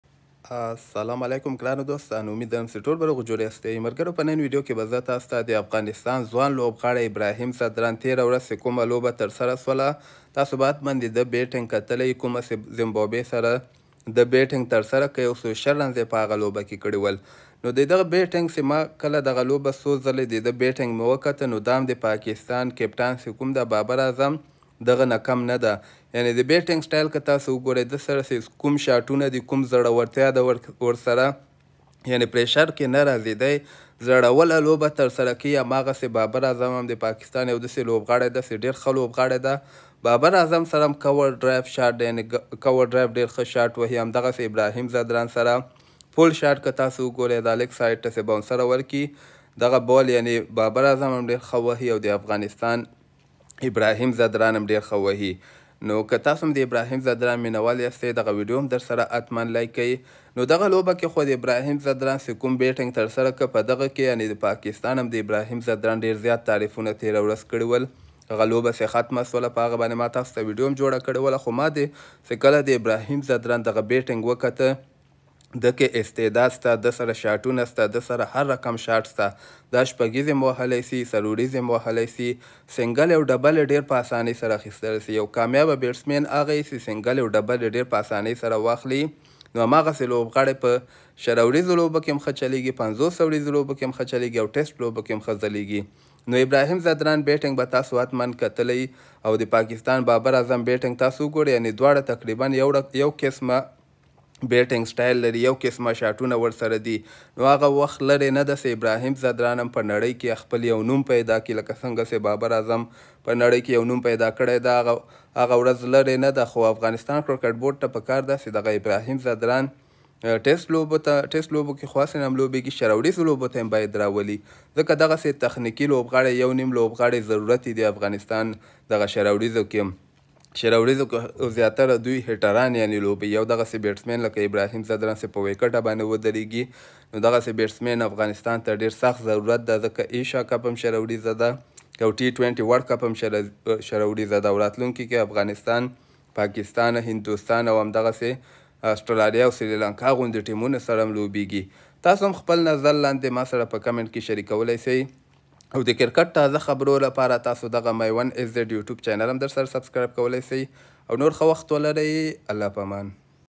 Babar Azam Like a Batsman From Afghanistan Ibrahim zadran || Maiwand sz
In this video I tell you about Pakistan cricket team captain babar azam like a batsman from Afghanistan by the name of Ibrahim zadran same action same style of you want to know more about it then watch complete video if you like it don’t forget to subscribe.